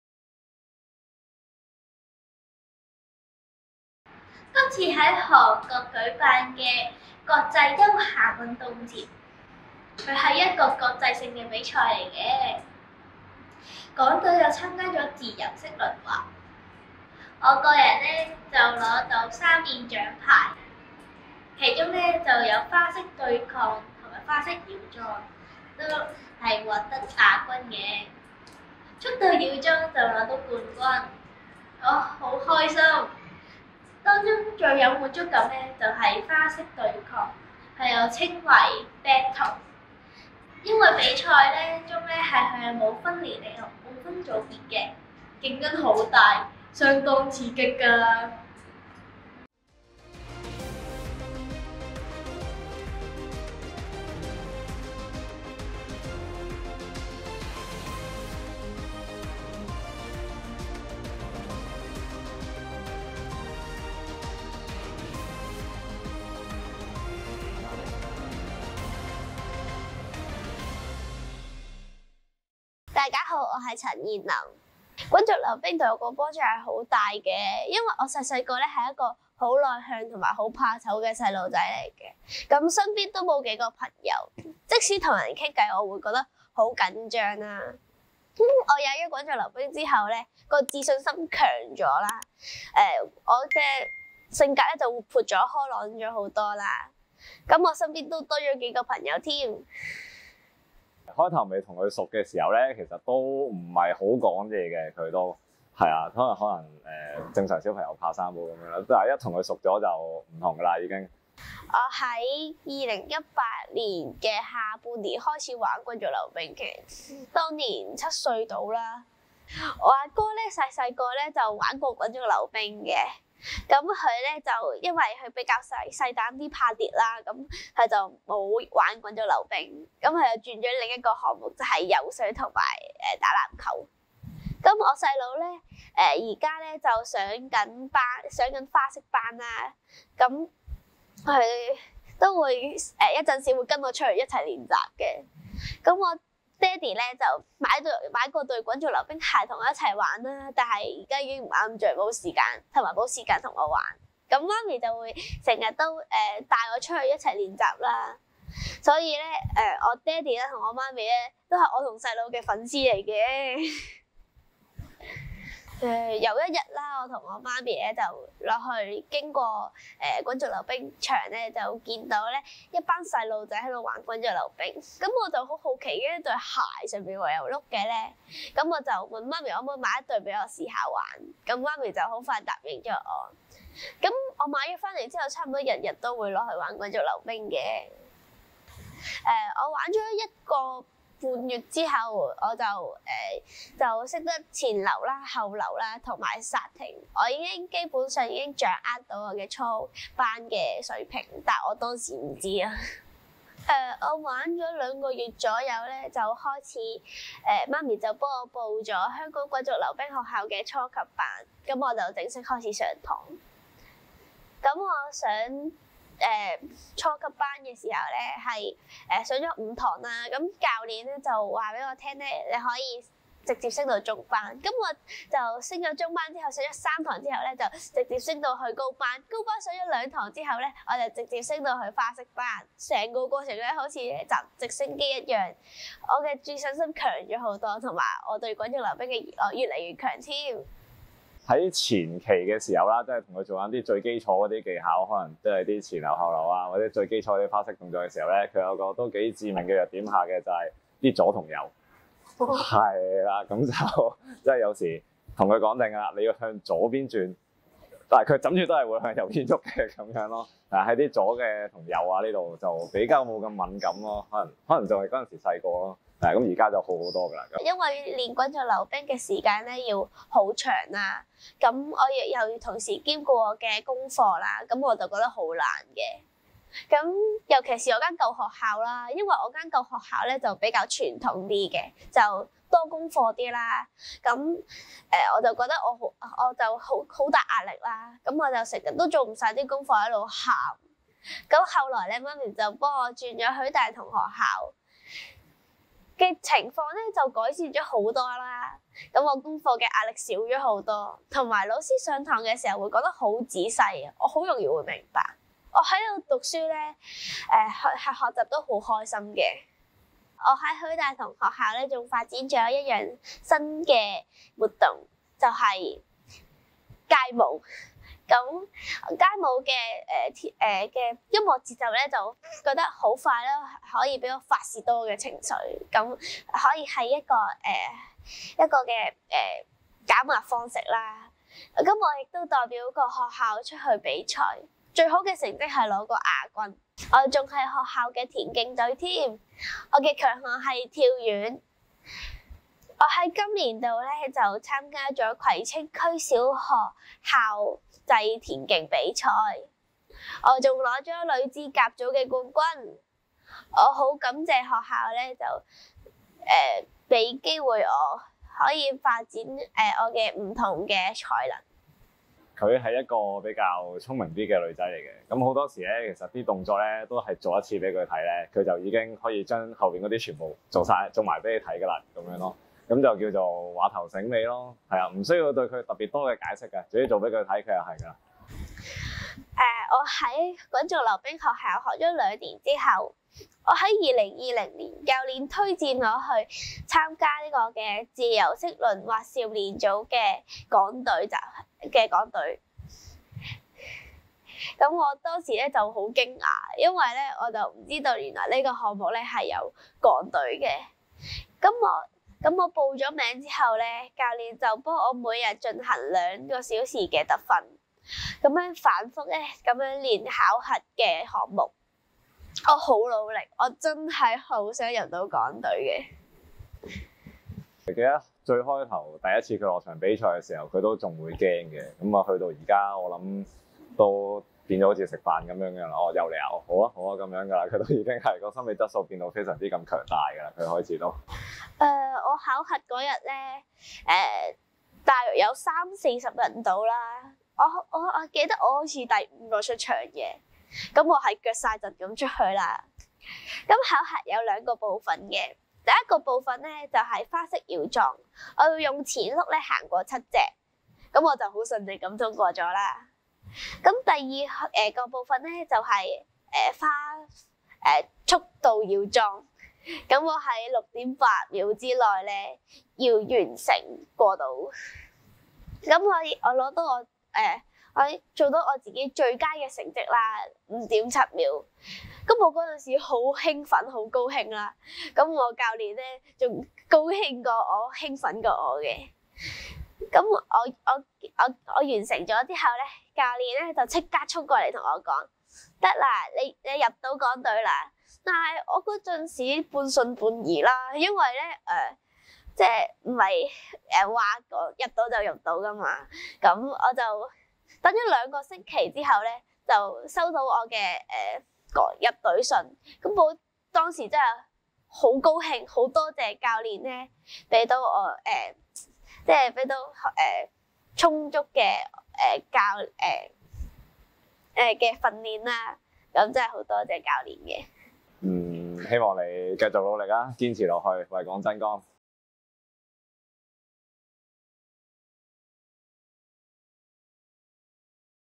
Good News23：2023年6月 6D班陳彥霖滾動青春夢──製作特輯
影片標題：《陳彥霖滾動青春夢》製作特輯 (2023)

6D班陳彥霖原是一位自小內向和害羞的小女孩，憑著玩滾軸溜冰運動增強自信心，更變得活潑開朗。

雖然遇上長達三年的全球疫情，也無阻她每天進行兩小時的特訓，最終成功投考「自由式輪滑集訓隊」。

在2023年期間，陳彥霖有幸代表港隊出戰「新加坡東盟自由式輪滑賽」和「韓國春川國際休閒運動節」，榮獲驕人成績。

在特輯中，很高興，能訪問彥霖，由她說出親身經歷，透過個人分享鼓勵大家敢於夢想，不怕困難，發揮潛能！

賽事成績榮獲：

新加坡東盟自由式輪滑賽
速度繞椿: 冠軍
花式刹停: 冠軍
花式繞椿:季軍

韓國春川國際休閒運動節2023自由式輪滑賽
少年女子組速度過樁賽:冠軍
少年女子組花式繞樁賽:亞軍
少年女子組花式對抗賽:亞軍

鳴謝製作本特輯人士：
學生說話訓練：黎寶燕主任
影片制作：鄧俊庭老師
核對畫面和說話內容：陳彥霖媽媽
接受訪問和借出場地：「香港滾軸溜冰學校」教練
其他：林湧先生